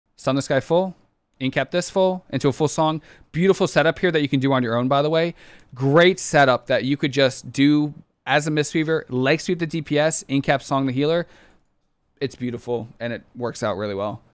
How to Setup Crowd Control as a Mistweaver!
Great way to setup crowd control as a Mistweaver. Even though we have limited CC, this setup can be done every minute!